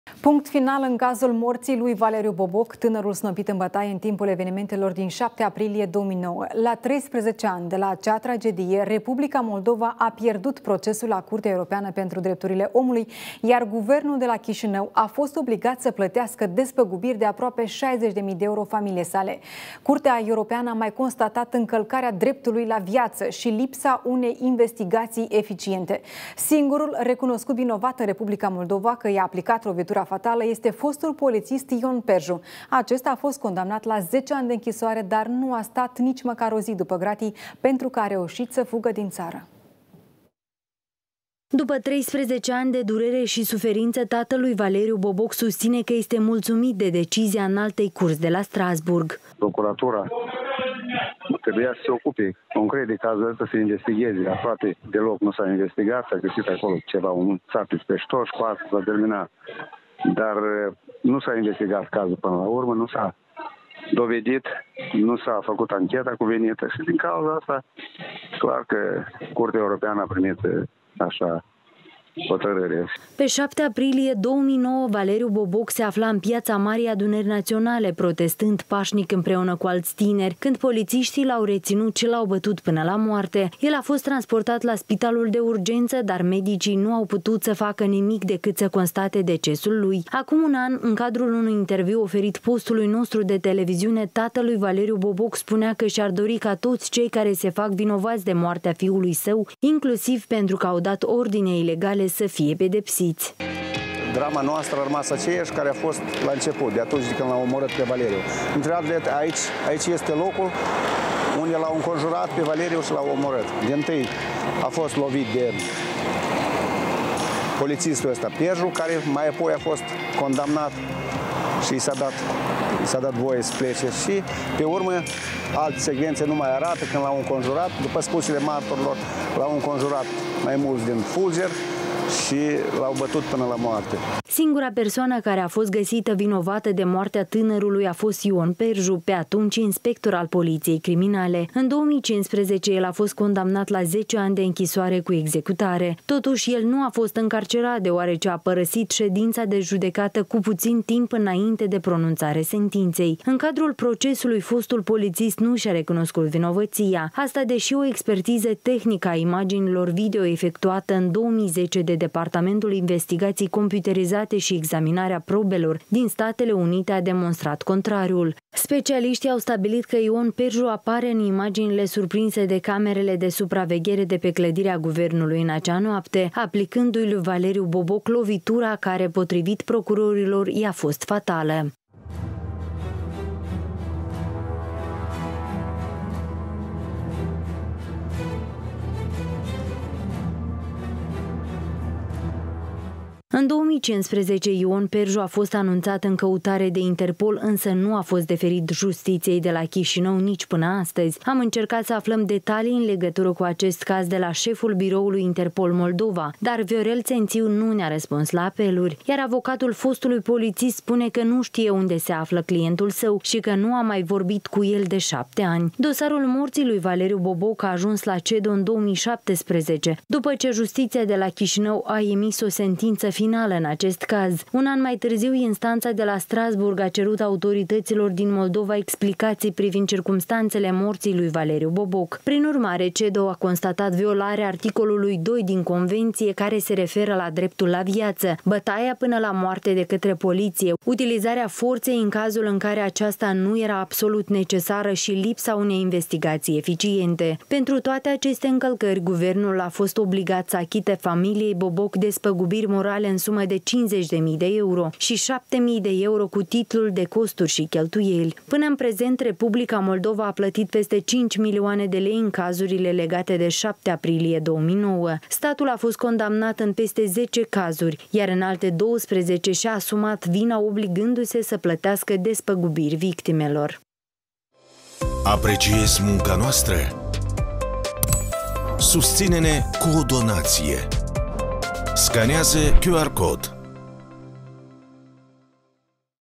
Și-a găsit dreptatea la CEDO. Guvernul a fost obligat să achite familiei Boboc aproape 60 mii euro
Punct final în cazul morții lui Valeriu Boboc, tânărul snopit în bătaie în timpul evenimentelor din 7 aprilie 2009. La 13 ani de la acea tragedie, Republica Moldova a pierdut procesul la Curtea Europeană pentru Drepturile Omului, iar Guvernul a fost obligat să plătească despăgubiri de aproape 60 de mii de euro familiei lui Valeriu Boboc. Înalta instanță a constatat încălcarea dreptului la viață și lipsa unei investigații eficiente. Singurul recunoscut vinovat că i-a aplicat lovitura fatală este fostul polițist Ion Perju. Acesta a fost condamnat la 10 ani de închisoare, dar nu a stat după gratii nici măcar o zi, reușind să fugă din țară.